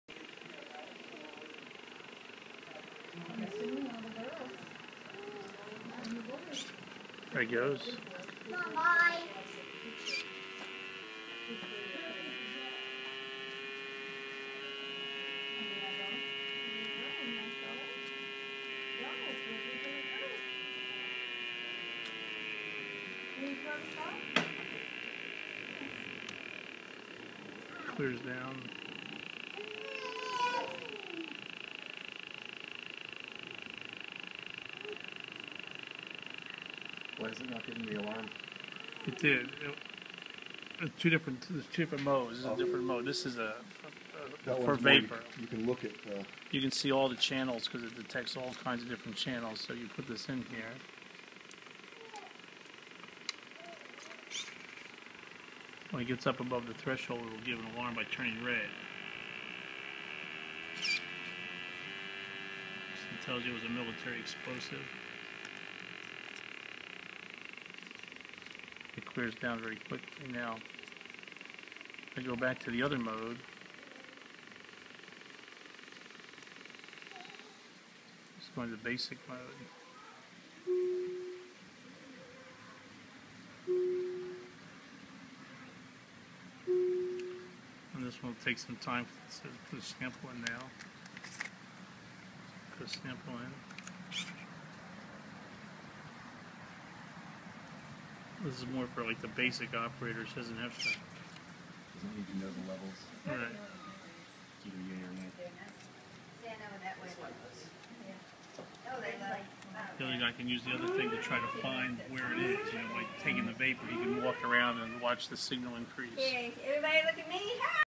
FLIR explosive detector
Early sales sample of FLIR explosive sampler/detector.

Hadn't read the user manual as it just arrived. Learning how to use it on the fly.